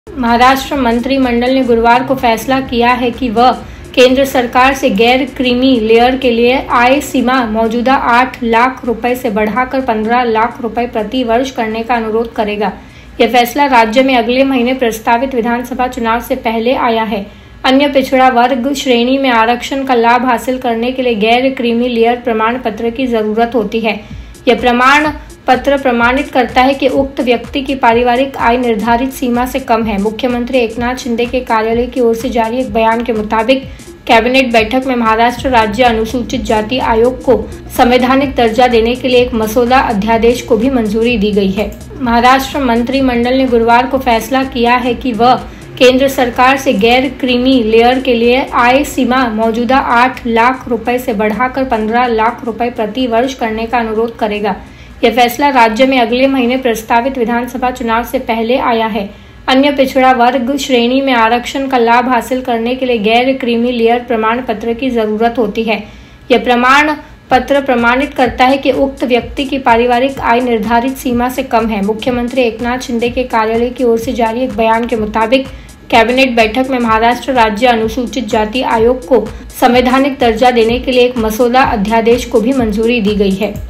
Shinde सरकार करेगी केंद्र से मांग, गैर क्रीमी लेयर की आय सीमा बढ़ाकर 15 लाख की जाए
महाराष्ट्र मंत्रिमंडल ने गुरुवार (10 अक्टूबर) को फैसला किया कि वह केंद्र सरकार से ‘गैर-क्रीमी लेयर’ (Non Creamy Layer) के लिए आय सीमा मौजूदा आठ लाख रुपये से बढ़ाकर 15 लाख रुपये प्रति वर्ष करने का अनुरोध करेगा. यह फैसला राज्य में अगले महीने प्रस्तावित विधानसभा चुनाव से पहले आया है. अन्य पिछड़ा वर्ग (ओबीसी) श्रेणी में आरक्षण का लाभ हासिल करने के लिए गैर-क्रीमी लेयर प्रमाणपत्र की जरूरत होती है.